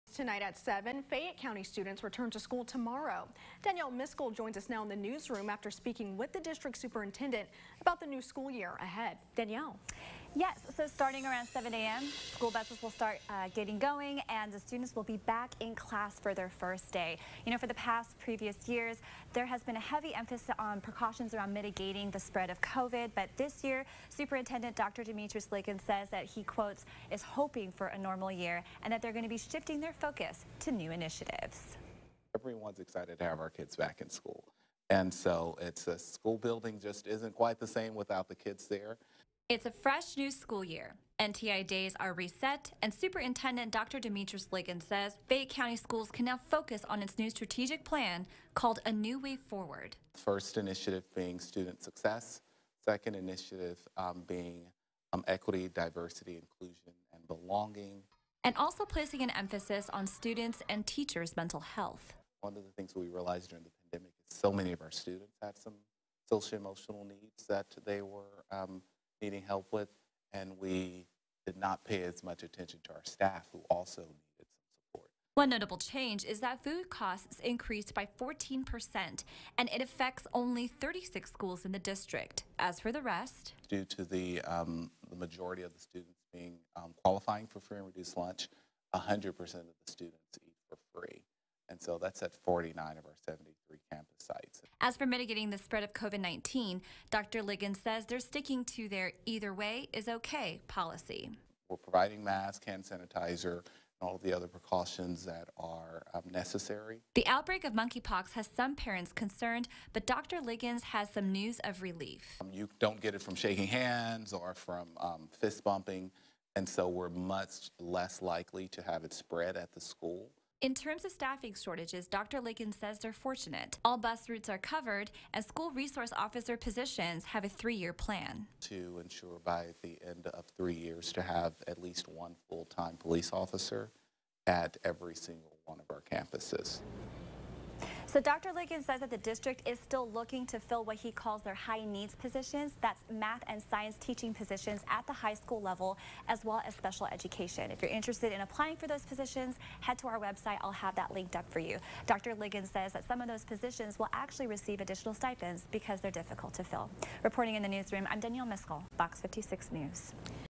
Fayette County Public Schools start tomorrow: Here's what's ahead
In less than 24 hours, school busses will get going and students will be back in class for their first day of school on Wednesday in Fayette County.